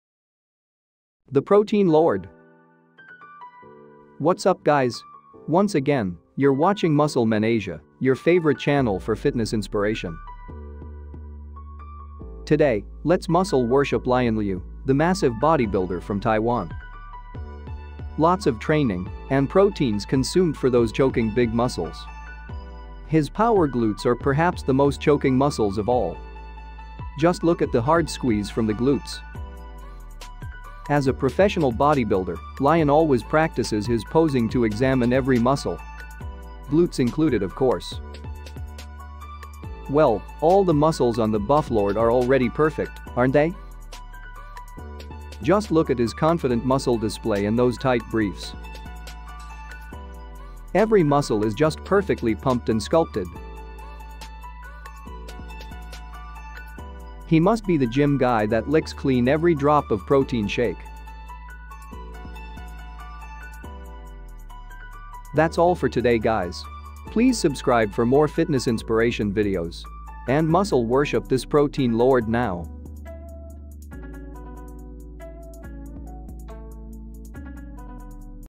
PROTEIN Lord To Muscle Worship! | Full Of Big Muscles
Lion Liu is choke full of muscles and proteins! Muscle worship this protein lord!

Buy a coffee to support us if you like our content

Model credits:
Lion Liu / IG:lion.ifbbpro